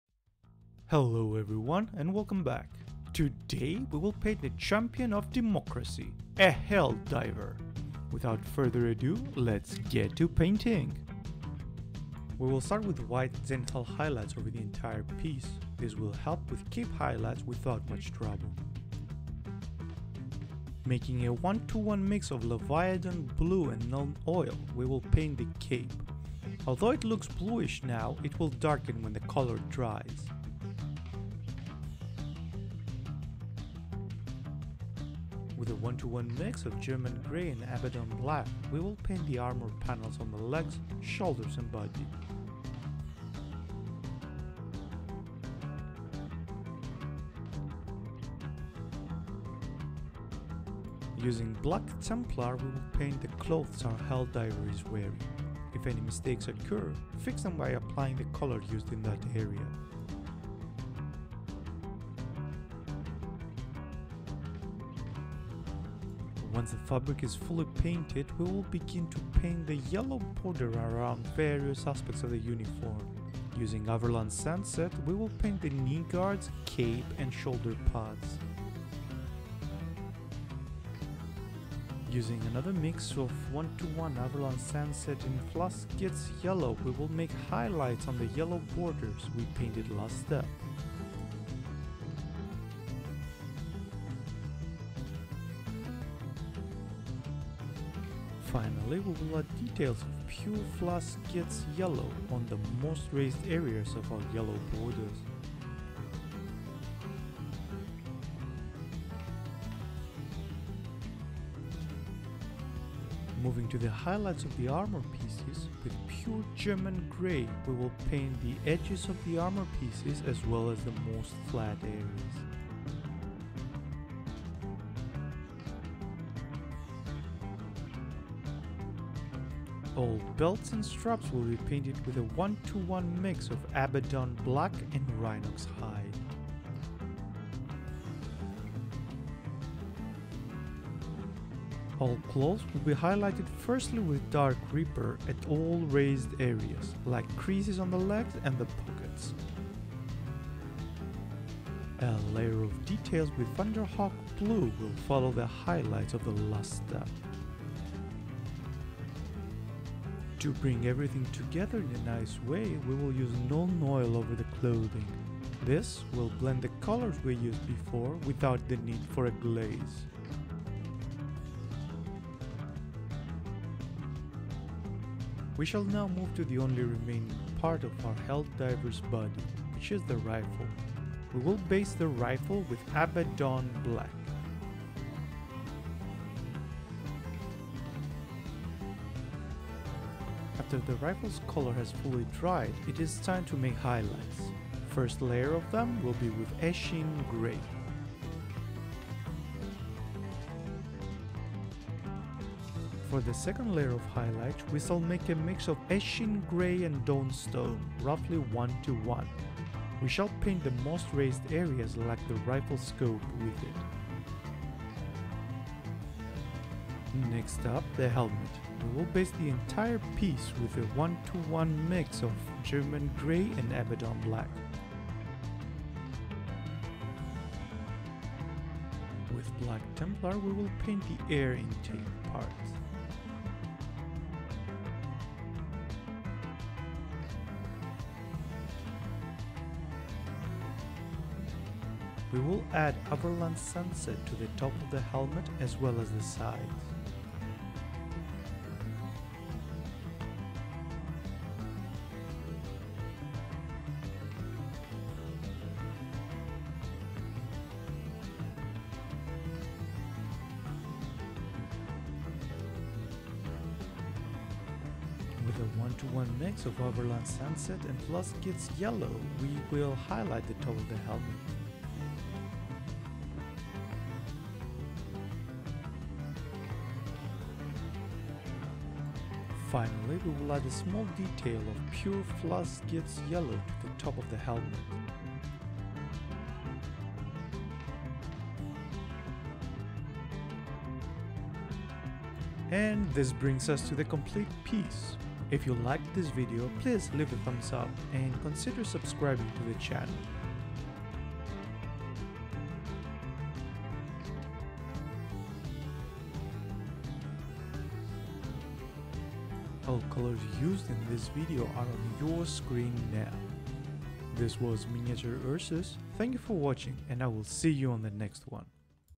Painting Together: Helldiver | Helldivers 2
Welcome, my democratic brethren, to another painting tutorial! In this video, we're diving into the thrilling world of futuristic warfare painting a champion that spreads Freedon, Liberty and Democracy.

Join us as we explore the intricate details of this elite soldier's armor, weaponry, and iconic helmet. Whether you're a seasoned hobbyist or new to miniature painting, this tutorial offers valuable techniques and insights to bring your Helldiver to life on the battlefield.

Throughout the tutorial, we'll guide you step-by-step through the painting process, from priming and basecoating to highlighting and detailing. Learn how to achieve stunning effects with layering and shading as we recreate the gritty realism of the Helldiver's universe.

Whether you're painting for display or gaming, this tutorial is sure to inspire and elevate your miniature painting skills. So gather your brushes, prepare your paints, and let's embark on this epic painting journey together!

And be sure to share your own Helldiver creations with us, we can't wait to see the elite troopers you bring to life on your tabletop!

For those of you who are interested in painting this awesome project, the link to the files are

All rights are reserved to the people who made the model and not to me, I found it a great model and thought of making a tutorial about it.

Happy painting !

~~Timestamps~~
00:00 - 00:12 Intro
00:13 - 01:20 Body Base Colors
01:21 - 02:54 Body Highlights
02:55 - 03:36 Rifle
03:37 - 04:50 Helmet
04:51 - 05:08 Showcase
05:09 - 05:19 Outro